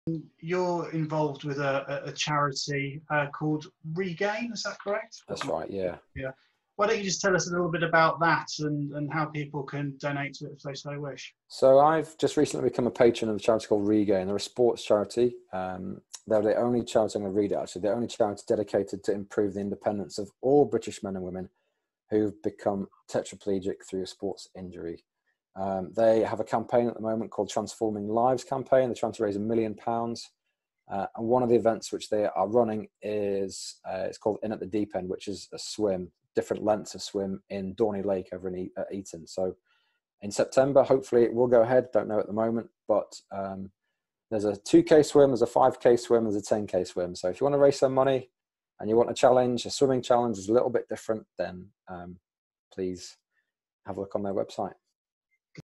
Ex-England and British & Irish Lions Rugby Player Charlie Hodgson talks about Regain Sports Charity
A clip from our "Establishing a High-Performance Mindset" Zoom webinar with ex England and British Lions Player Charlie Hodgson and Team GB Wheelchair Rugby's Aaron Phipps.

In this clip Charlie talks about Regain Sports Charity, who he is patron of.

Please do not use this video without permission